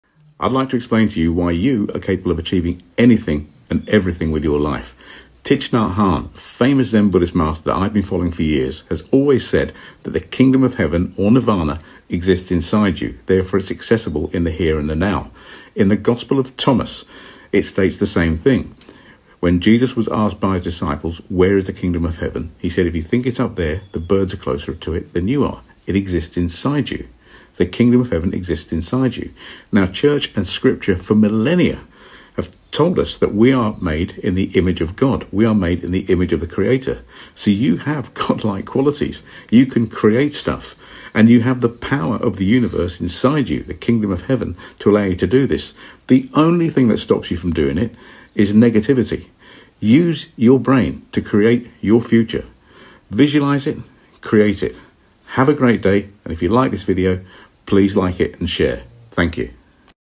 You Can Achieve Anything Because You Have Universal Power
The Kingdom of Heaven/Nirvana exists inside you so it available in the here and now.

You are also made in God/the Creator's image.

God created everything and is omnipresent.

This means that you are a creator (because you are made in God's image).

And you have universal power to create your life the way you want it to be (because you have the Kindgom of Heaven/Nirvana inside of you).

The only thing that is stopping you is negativity, and lack of self-belief.

Have faith in yourself and your universal/God-like ability.